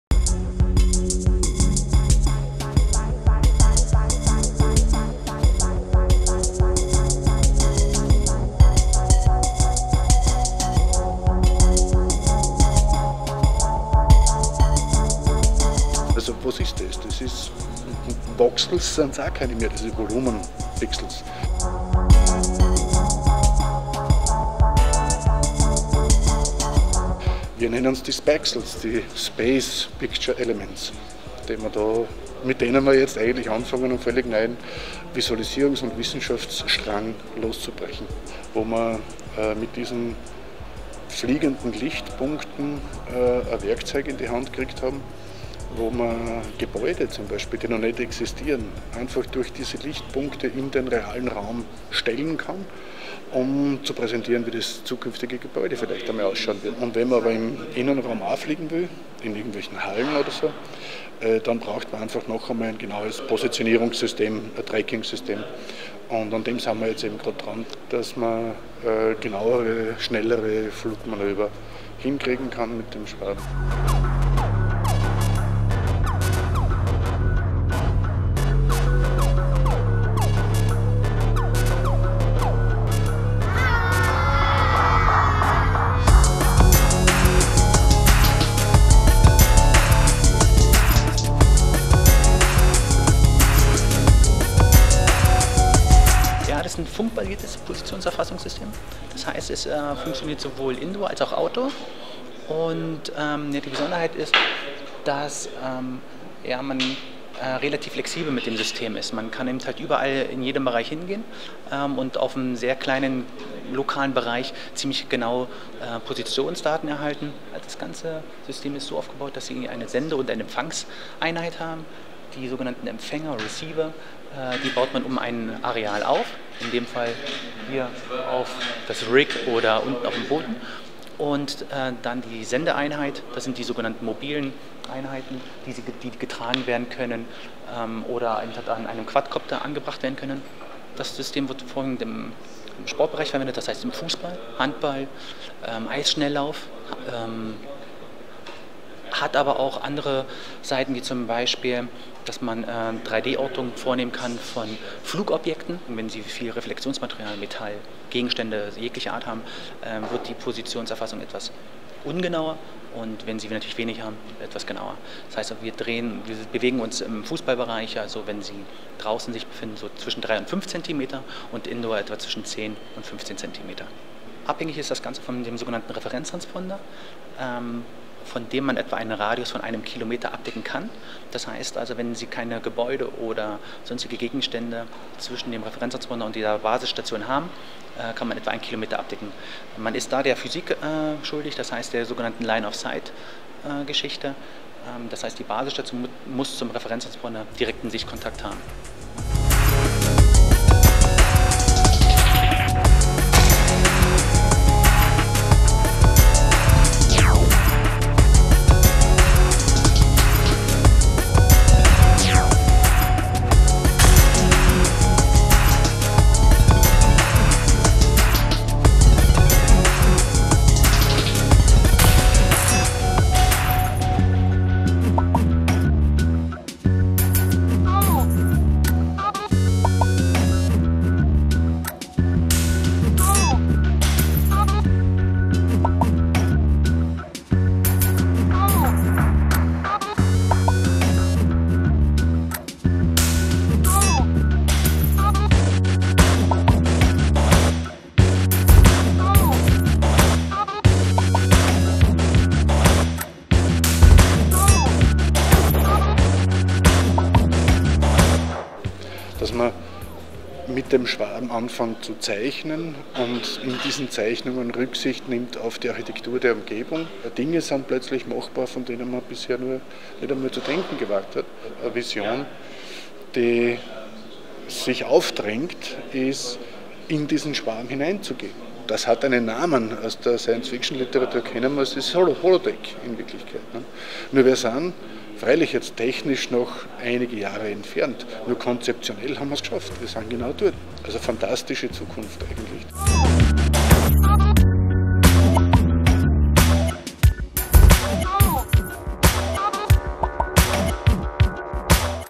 Ars Electronica Futurelab Linz, Quadrocopter, local position measurement by Abatec
Die Quadrocopter hatten bei der voestalpine Klangwolke ihren ersten Auftritt. Jetzt geht die Reise weiter, es wird entwickelt, geforscht, optimiert, Zukunftsmusik wird komponiert. Die Steuerung der Quadrocopter über GPS war für den Aussenraum toll, um auch Shows im Innenraum fliegen zu können muss aber was anderes her - local position measurement nennt sich ein System, das von der Firma Abatec entwickelt wurde und jetzt an unseren "Spaxels" getestet wird. Weitere Infos über die Spaxels finden Sie

The quadrocopters had their first big performance during the voestalpine Klangwolke 2012. Now the journey continues, the next steps are ahead. GPS is great for positioning when you're outdoors, if you want to fly indoor, you need something else. lpm (local position measurement) by Abatec is the key for to the evolution of the Spaxels.